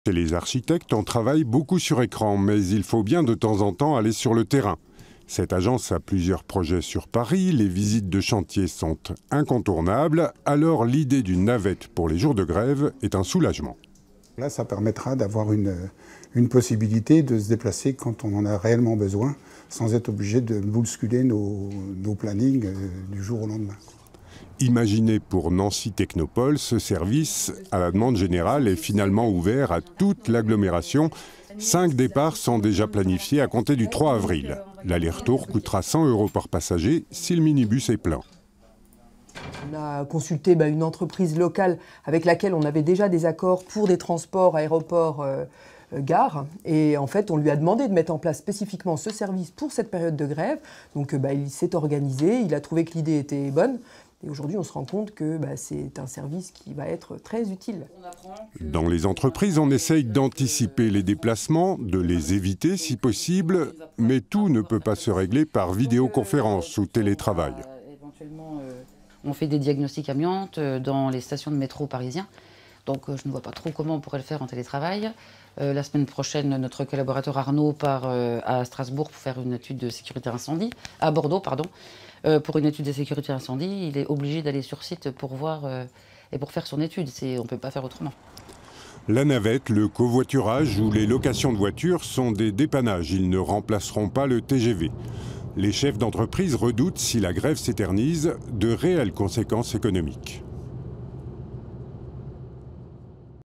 Grève de la SNCF. Le technopole de Nancy-Brabois a mis en place un système de navette
Le 3 avril prochain débutera la mobilisation des cheminots contre la réforme de la SNCF.
La grève devrait avoir des conséquences sur l'ensemble des secteurs de l'économie française. Pour réduire au maximum l'impact du conflit sur leur activité, les entreprises s'organisent.
Exemple sur le technopole de Nancy-Brabois où l'on a mis en place un système de navette.